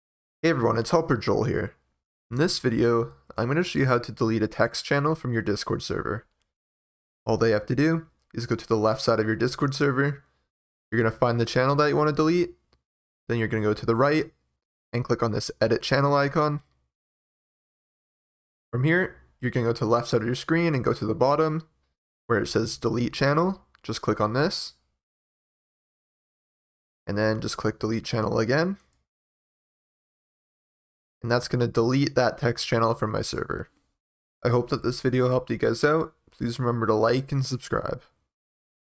Delete A Text Channel In A Discord Server
In this tutorial I show you how to delete a text channel from your Discord server. Removing a text channel in your Discord server is quick and easy to do.